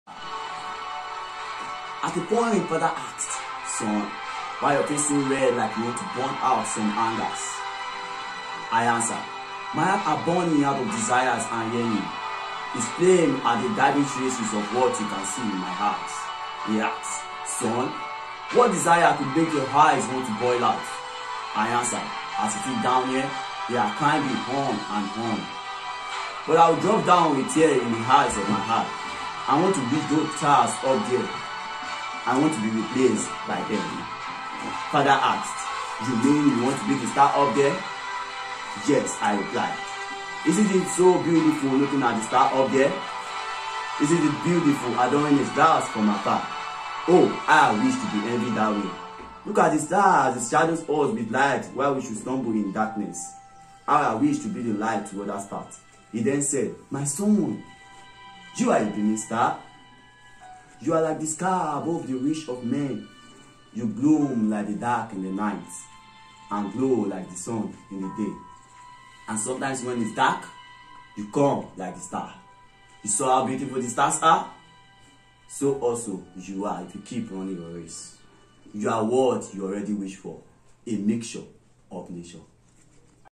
A Beaming Star by Taiwo Odunayo Bayode
Poet’s Biography
Taiwo O. Bayode is a law students, a Poet and a Writer. He develops enthusiasm in writing, particularly poetry. He has one of his works published in MyWoven Poetry Magazine, also a one time winner of the Golden award for the TTF (Tony Tokunbo Fernerdes) online poetry international competition for 2022. Lot of his works can be seen in his Instagram page, his handle; Blessedpen_.

I had a high dream of becoming something very great and having significance, this I wanted to start by climbing on the next place in my life, but the thorns I face were so unbearable, my father saw how discouraged I was, he told me somethings that led to the poetry...

Poem Text
At a point, Father asked me
"son, why your face so red like
you want to burn out some angers?"
I answered, "my heart are burning out
of desires and yearning,
it's flames are the diving traces of
what you see in my eyes"

he asked, "son, what desires could
make your eyes want to boil out?"
I answered, "as it is down here,
they're climbing on and on,
but I will drop down a tear in
In the ice of my heart, I want
to reach those stars up there with
my bare hands, I'm so jealous of them
that I want them be replaced by me..."

"you mean you want to become
the stars we looking at?" he asked,
I answered, "yes! isn't it beautiful adoring its glance from afar? Oh! how I wish to
be envied that way, look at the stars
as it shadows us with light while
we should stumble in darkness,
how I wish to be light to others path"

he then said, "my son, part of you
has a beaming star, you are like
the sky above the reach of men,
you gloom like the dark in the night,
and glow like the sun in the day, and
sometimes when it is dark,
you come like the stars,
you saw how beautiful the stars are?
so also would you be if you keep
running your race & raise up your ego,
you are what you already wished for,
a mixture of what nature holds. "